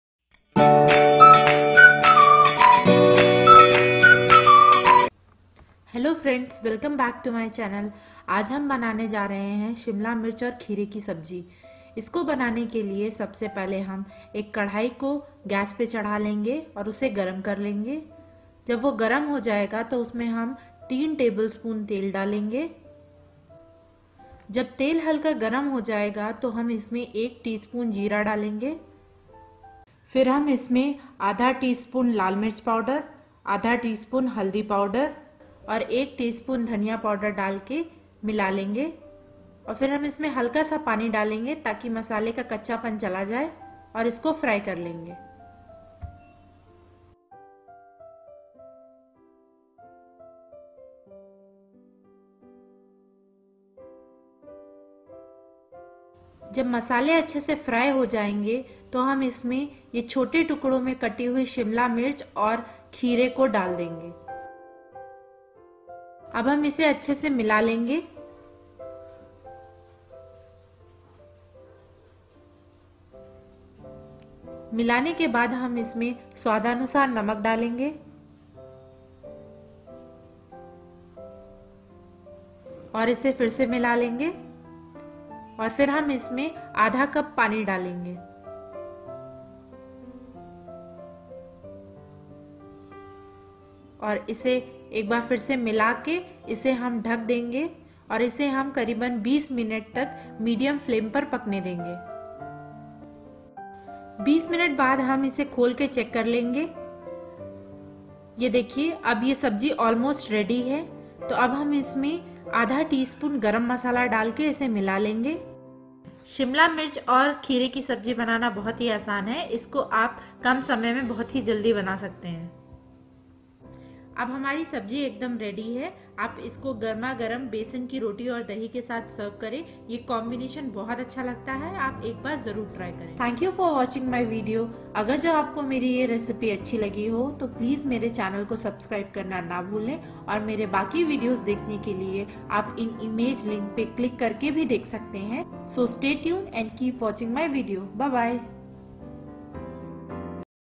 Kheera aur Shimla Mirch ki Sabji | Cucumber Capsicum Curry | No Onion Garlic Sabji
1 Cucumber
2 Capsicum
3 tbsp Oil
1 tsp Cumin Seeds
1/2 tsp Red Chilli Powder
1/2 tsp Turmeric Powder
1 tsp Coriander Powder
1/2 tsp Garam Masala